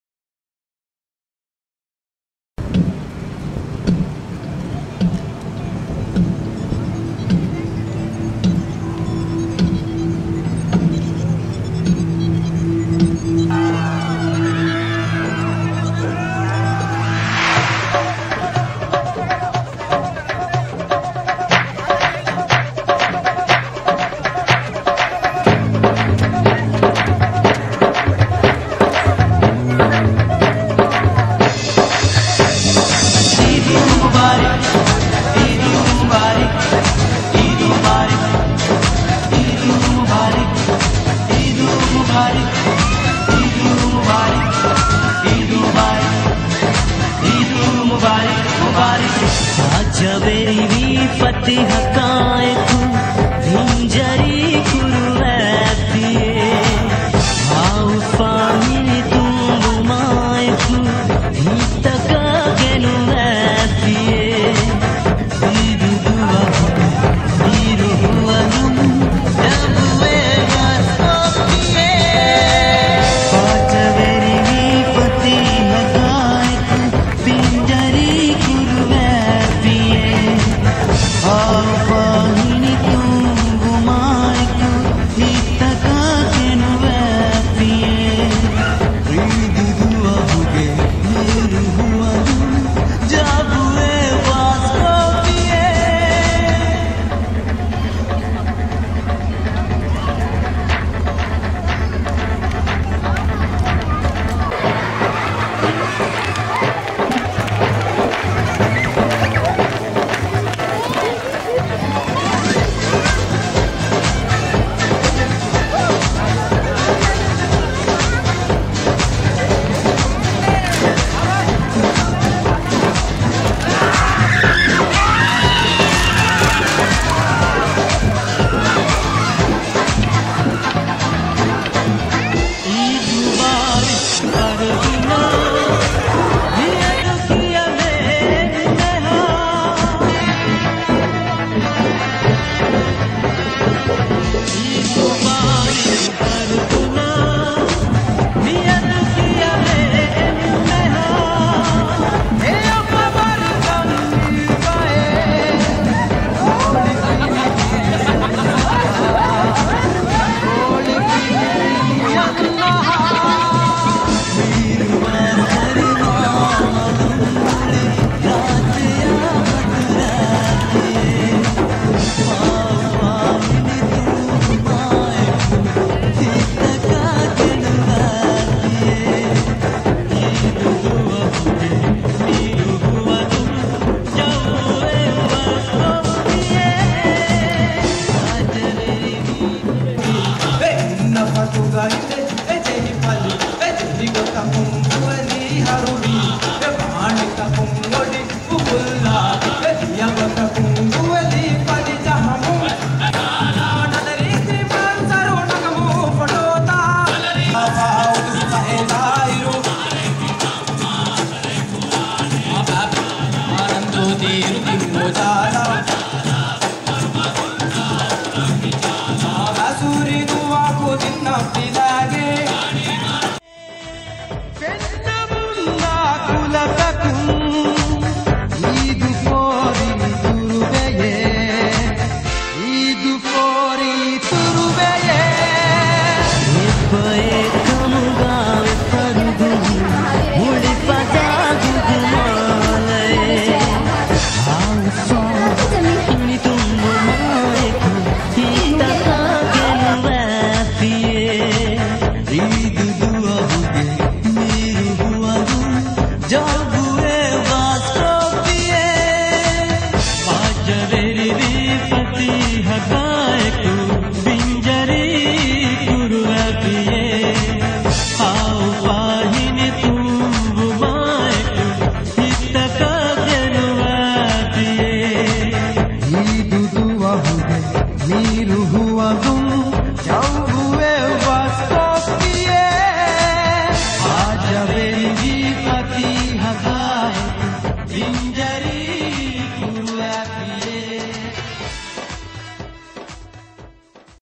Maldivians celebrate Fitr Eid
Maldivians celebrate Fitr Eid in the capital Male.